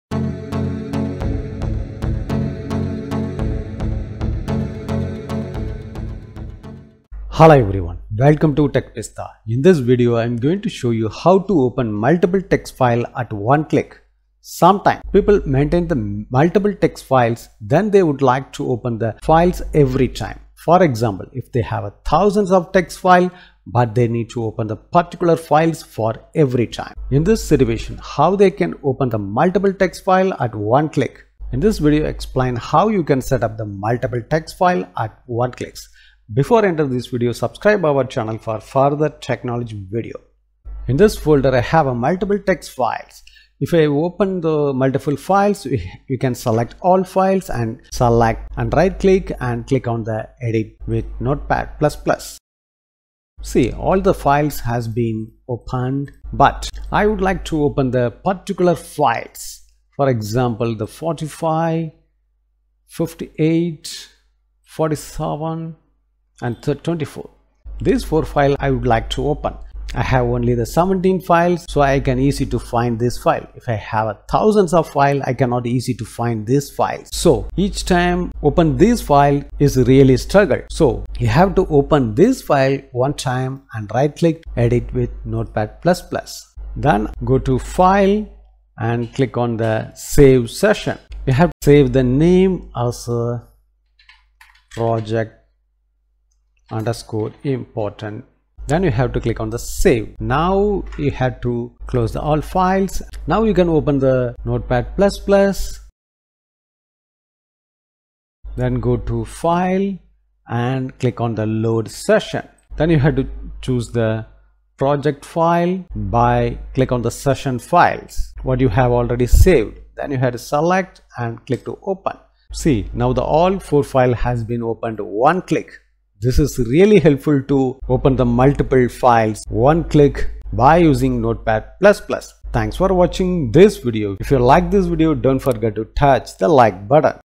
How to open multiple text files at one click?
Sometimes people maintain the multiple text project files, then they would like to open all files every time. For example, if they have 1000 text project files, but they need to open the particular files for each time. In this situation how they can open multiple text files at one click.

In this video explains how you can open multiple text files with one click.